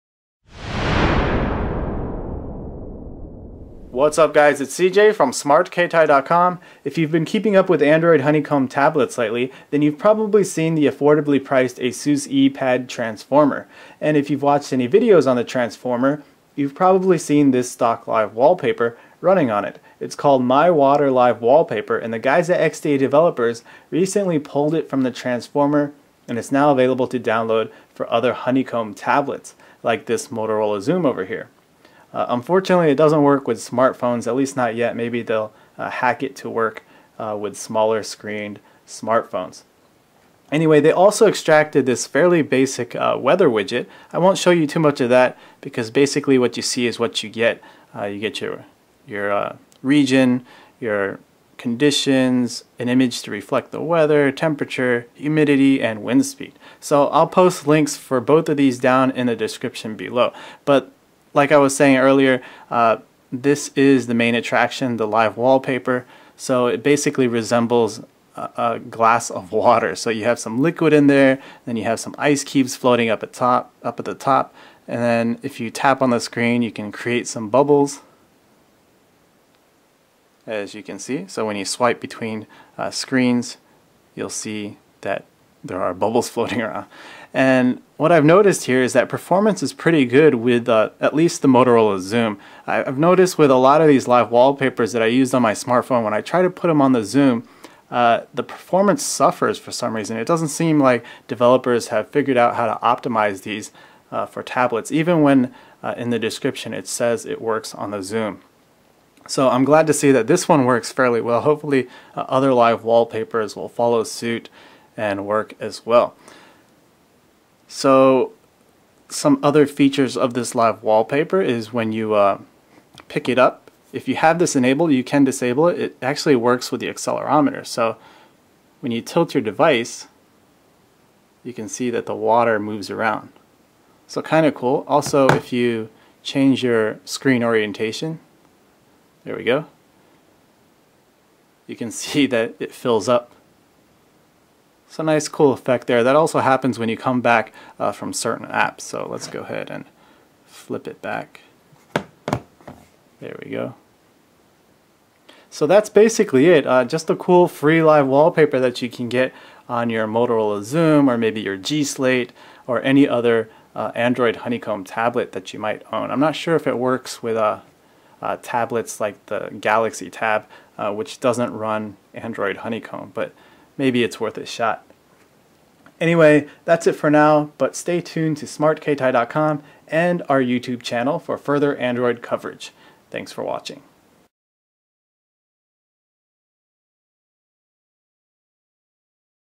ASUS Transformer Water Live Wallpaper running on Motorola Xoom, get it for your Honeycomb tablet
In this video, we check out "My Water" live wallpaper for Android 3.0 Honeycomb tablets. This wallpaper comes stock on the ASUS Eee Pad Transformer, and the folks at XDA recently pulled it from the device for use on all other Honeycomb tablets. We show you the LWP and a weather widget running on a Motorola Xoom.